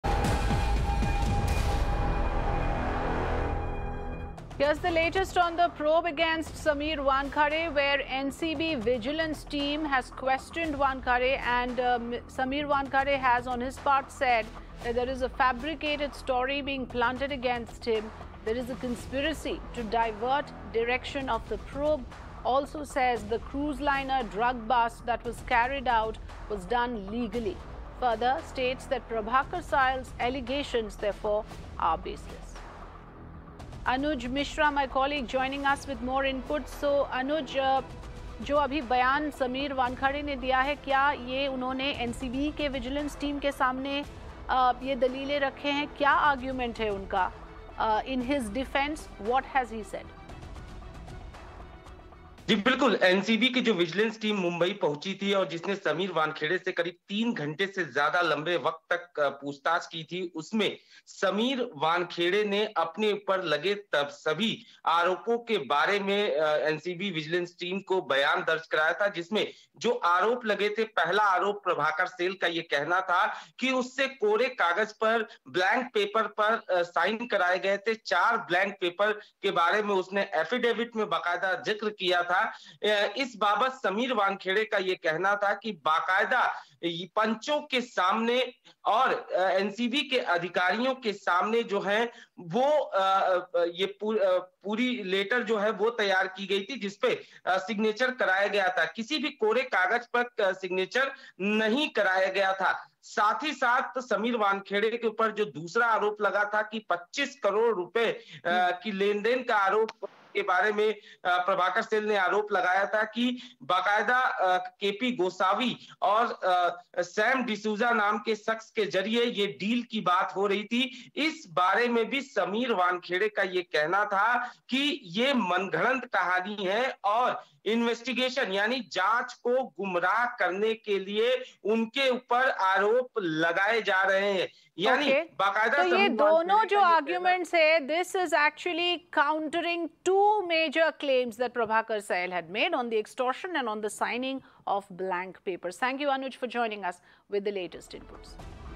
Drug bust was carried out legally: Sameer Wankhede on being questioned by NCB
NCB vigilance team questioned the Zonal Director of Narcotics Control Bureau (NCB), Sameer Wankhede. The NCB team questioned Sameer Wankhede for more than 3 hours, where he clearly denied all the allegations against him. Sameer Wankhede says that " it is a fabricated story planted against me and it is a conspiracy to divert the direction of the probe." Wankhede also said that "the cruise drug party bust was carried out legally." He further claims that NCB's independent witness, Prabhakar Sail's allegations are baseless. Sail had claimed that on the night of the drug bust he was made to sign a blank affidavit.